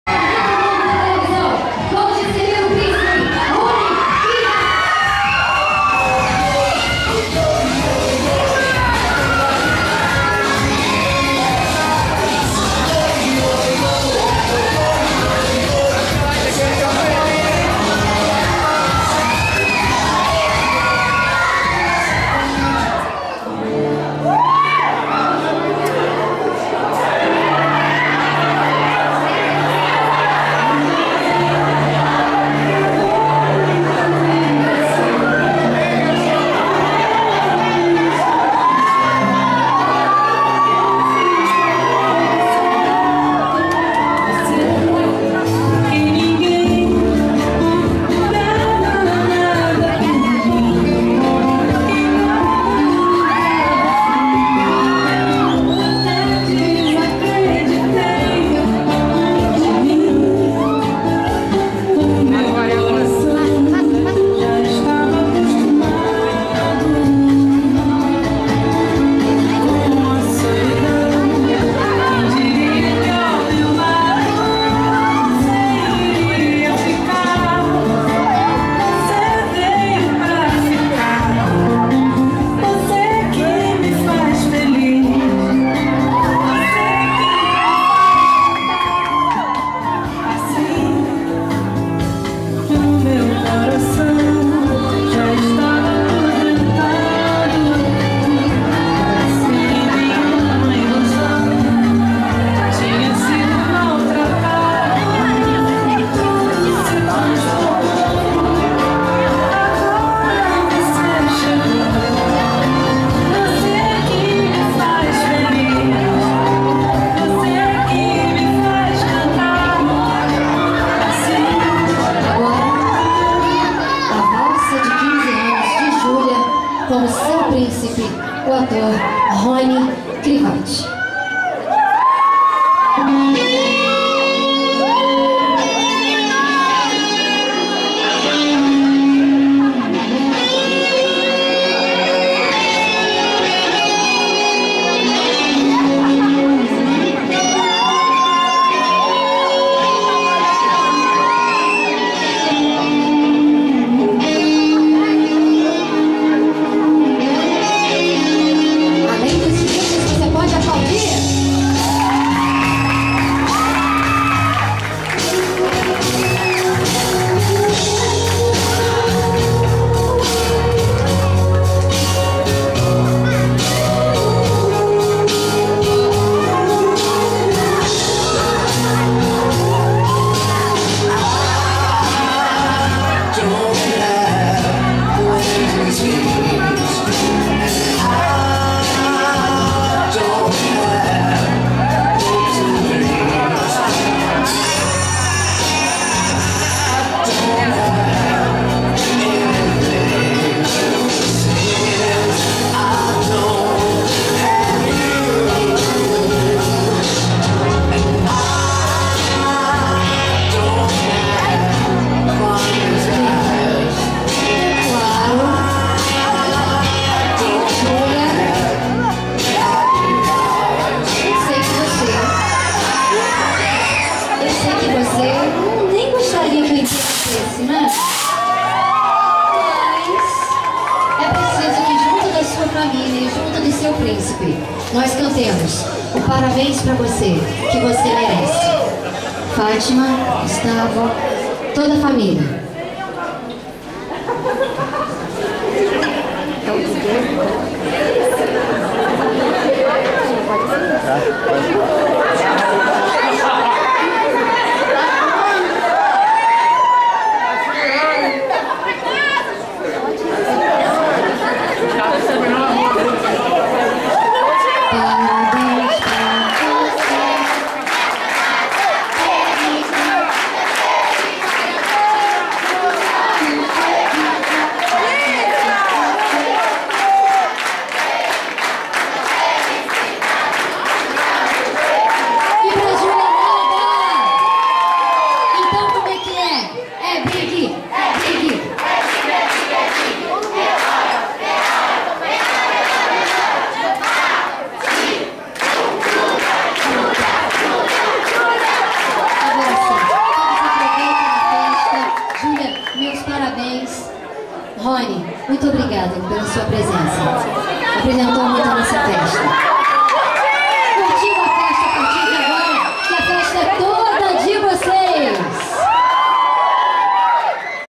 **ATOR RONNY KRIWAT-PRÍNCIPE DE JÚLIA ANDRÉIA-BY JOICENY BAZETH**
ATOR GLOBAL ENCENOU COM SUCESSO SER PRÍNCIPE DE 15 ANOS! MUITO SIMPÁTICO E LINDO AO EXTREMO!